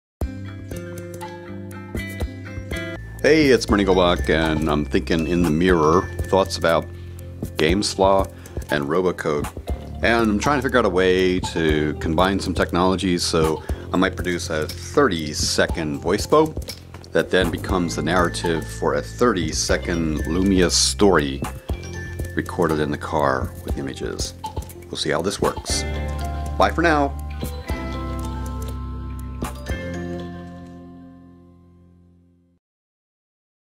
30 Second Storytime
Laying a short VoiceBo under a Lumia Storyteller video to let people see where I'm talking.